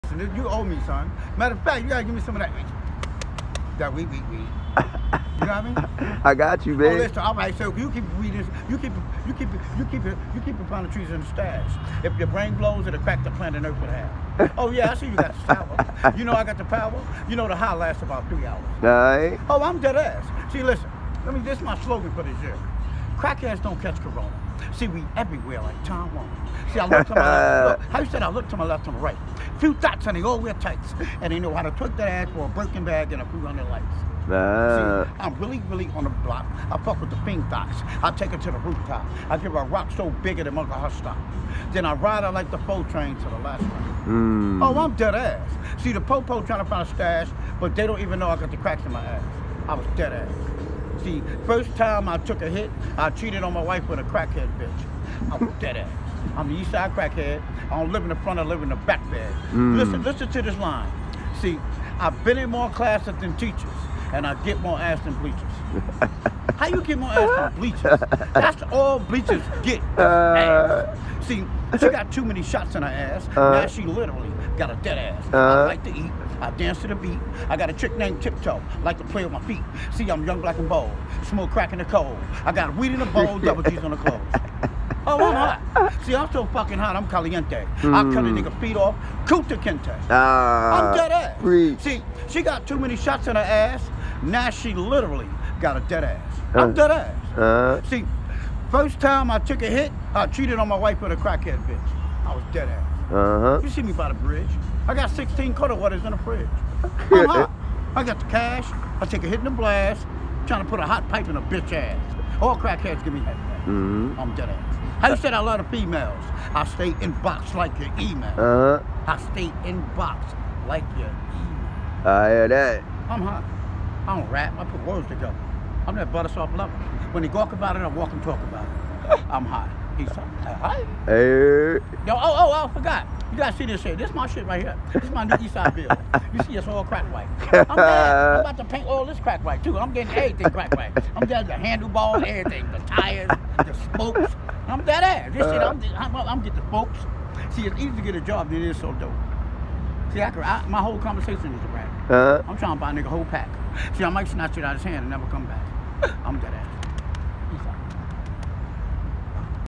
Cast Vallure listening to Eastside Crackhead spit some bars Pt2.
Cast Vallure 2020 vlog footage in Harlem at the field.

Cast Vallure getting a rap from that nigga Eastside Crackhead.